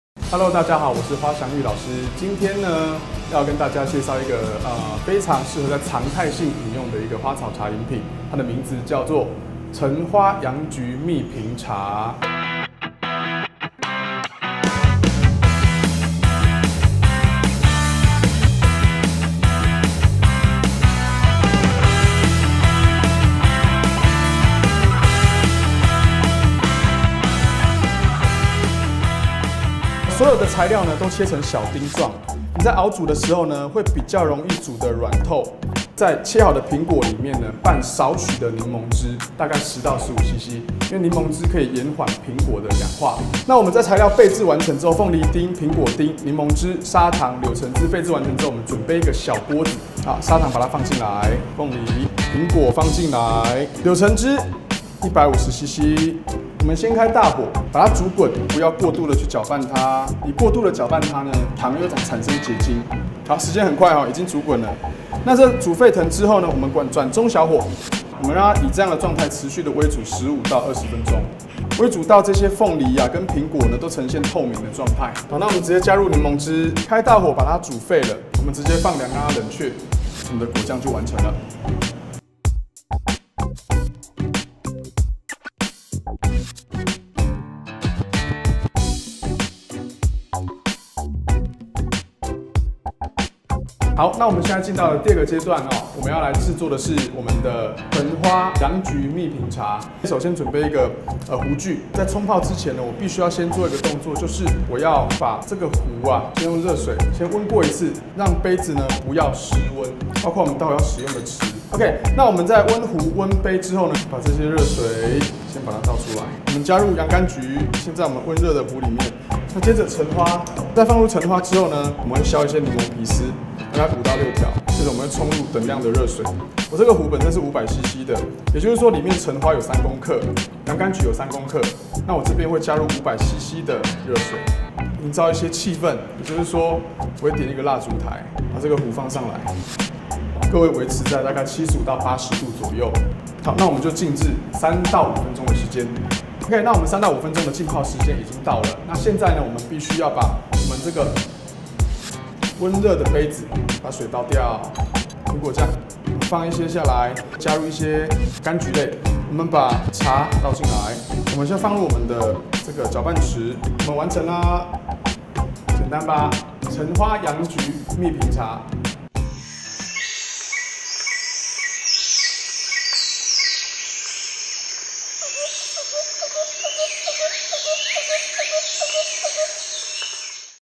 [5分鐘教室] 果醬花茶新搭配！橙花洋菊蜜蘋茶
時序進入五月，有點熱又不是很熱、有時又有點涼意，想要養生點，少喝冰品，還是喝杯溫暖又不上火的花茶，這一次邀請飲品職人--花祥育老師，教我們一款老少咸宜，無咖啡因、無酒精的橙花洋菊蜜蘋茶，可隨著季節時令水果自行搭配調整口味，學一款花茶，四季通用！